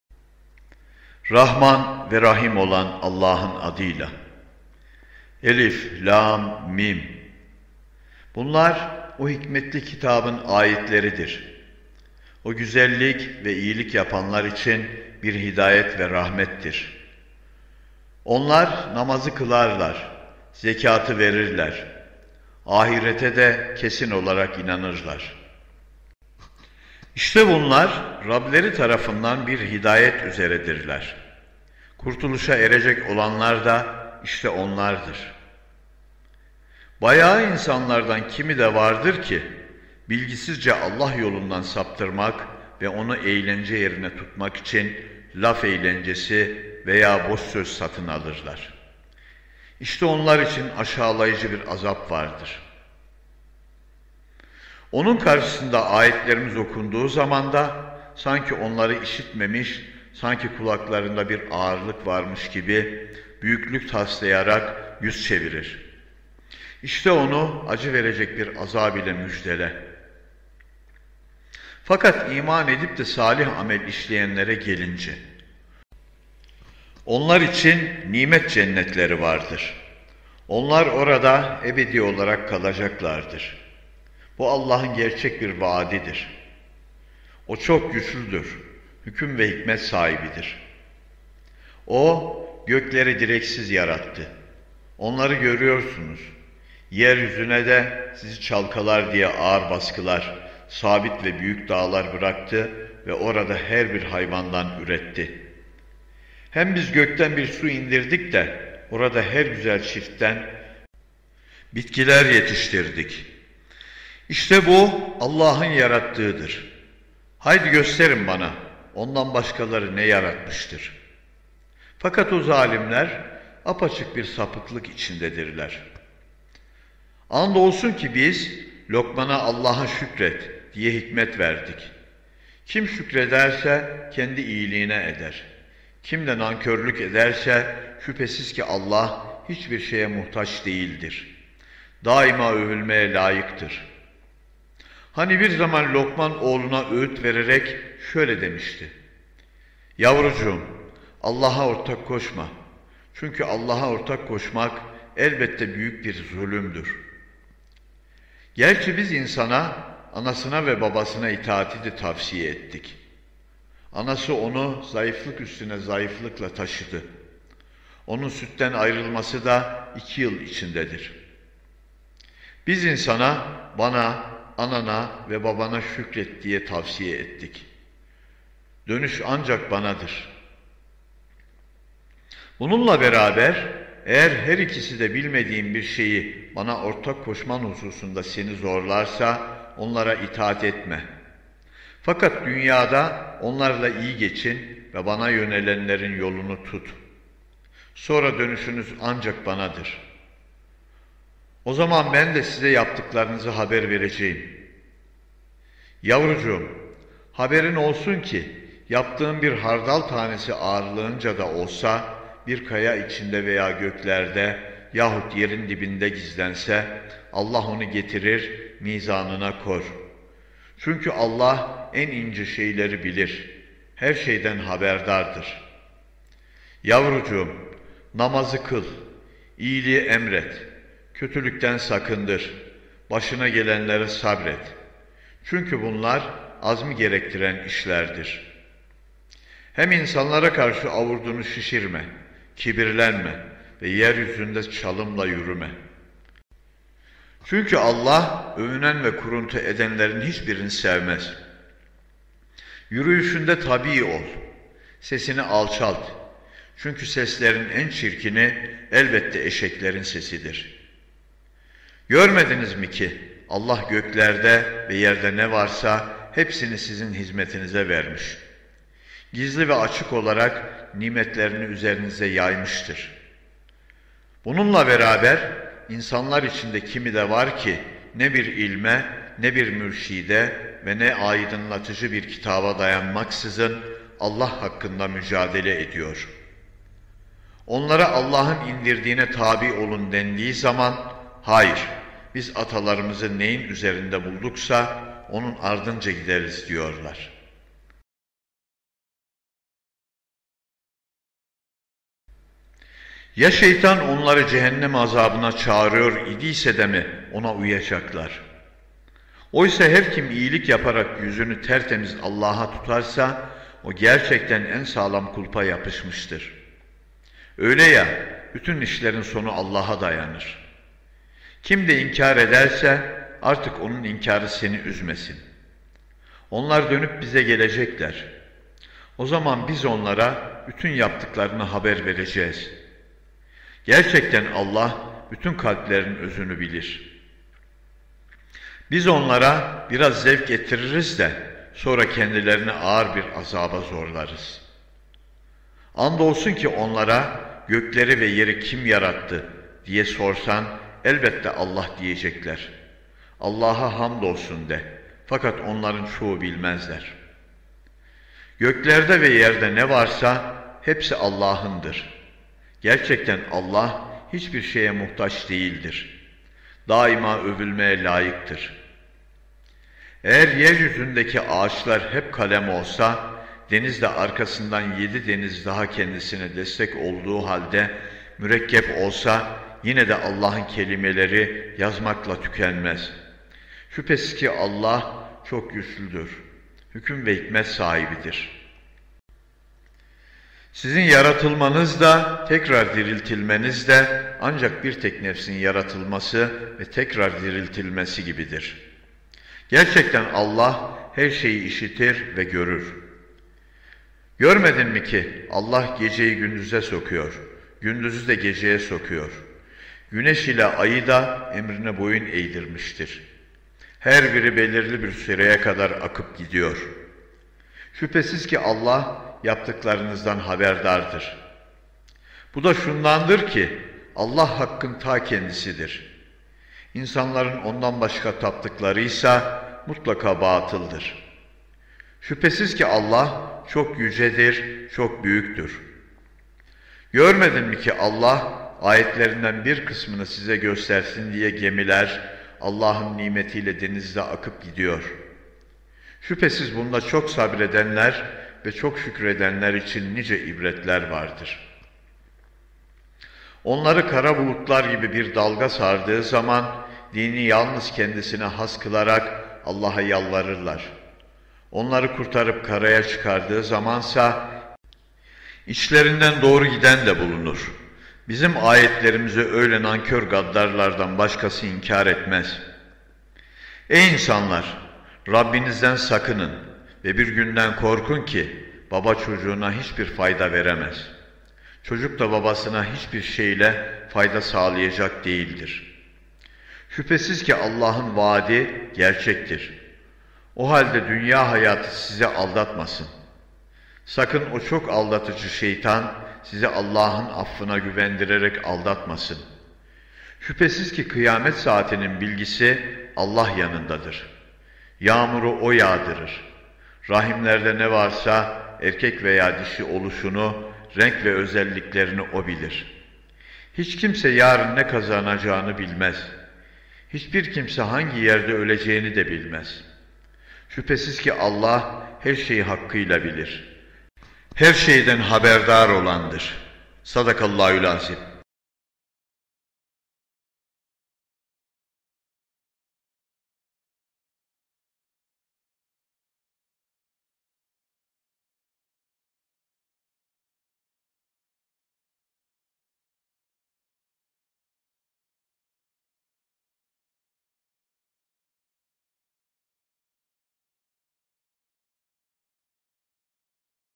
LOKMAN SURESİ MEALİ DİNLE KUR ANİ KERİM MEALİ FULL
KUR AN MEALİ  LOKMAN SUREİ ŞERİFİ TÜRKÇE AÇIKLAMASI SESLİ NÜZUL SIRASINA GÖRE
TAMAMI TEK PARÇA HALİNDE SESLENDİRME
OKUYAN SESLENDİREN
YILDIRIM ŞİMŞEK